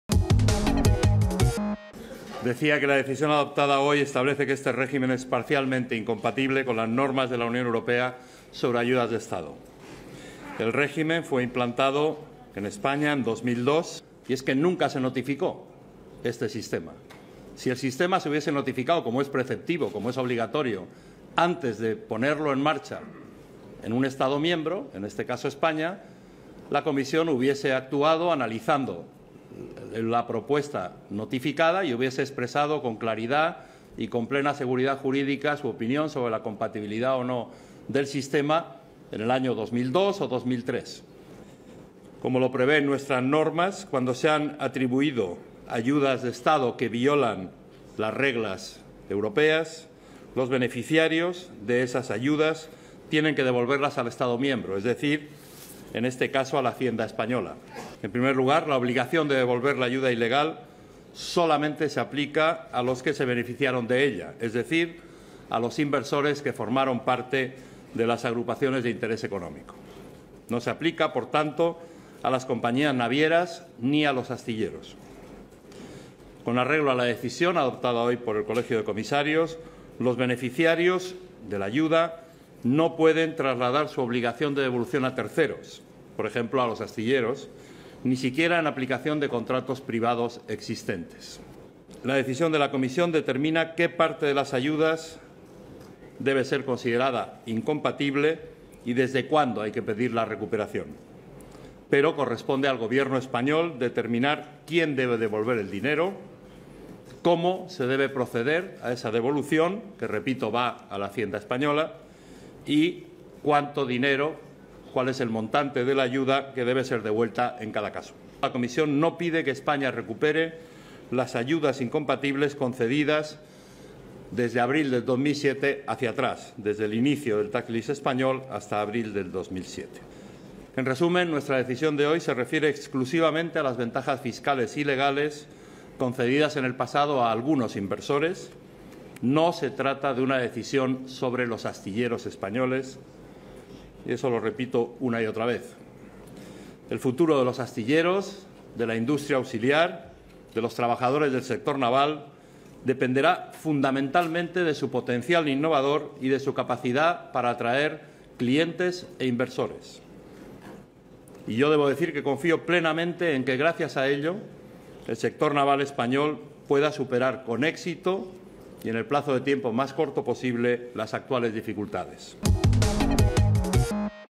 Almunia anuncia que tendrán que devolver las ayudas recibidas desde 2007
La Comisión Europea ha pedido este miércoles sector naval español que devuelva parte de las ayudas recibidas entre 2007 y 2011, calificadas de "ilegales" por Bruselas. Las subvenciones deberán devolverlas los inversores que financiaron la construcción de los buques, pero no los armadores ni los propios astillero.